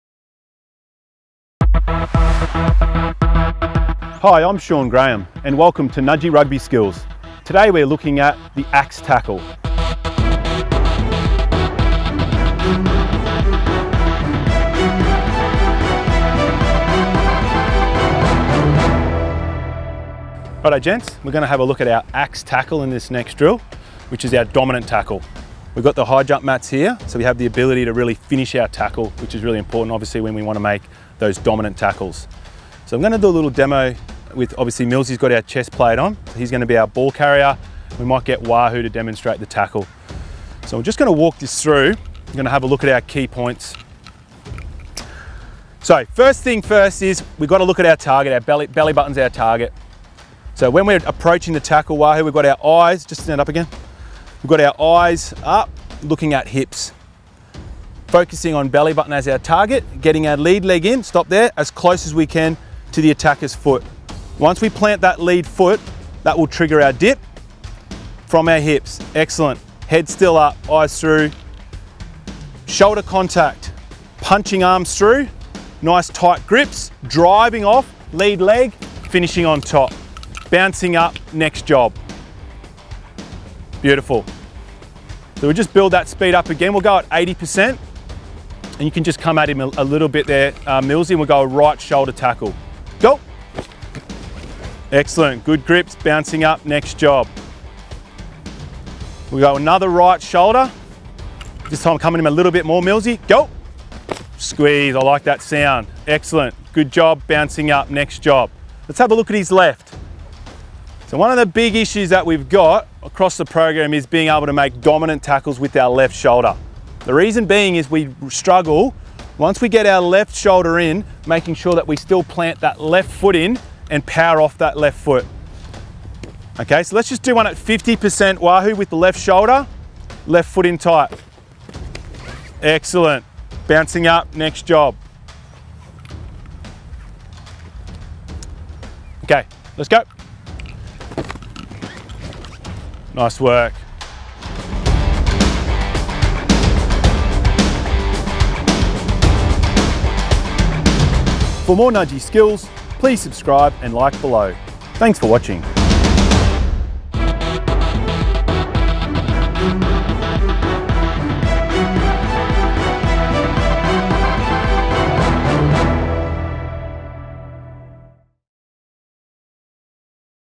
Nudgee Rugby Skills - The Axe Tackle
Master the technique to make dominant (axe) tackles on both shoulders. Key pointers:
- Drive off lead leg in tight which triggers dip
- Shoulder contact, head in tight and eyes remain up
- Belly button target
- Punch arms through with tight grips